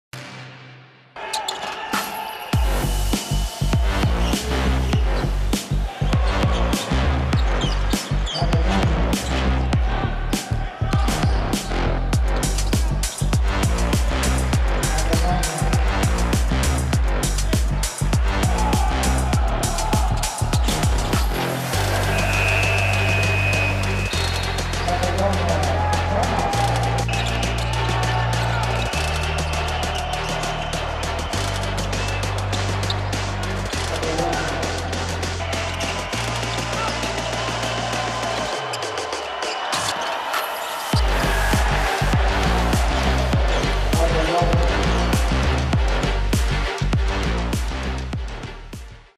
Mike Daum VS Allianz Pallacanestro Trieste
Esordio da sogni per il Bertham Derthona Tortona che vince la sua prima gara ufficiale in Discoveryplusit Supercoppa presented by UnipolSai al suo esordio!
Dominante la prestazione di Mike Daum con i suoi 2️⃣3️⃣ punti in 2️⃣4️⃣ minuti tirando con il 7️⃣0️⃣% da 2 e catturando 7️⃣ rimbalzi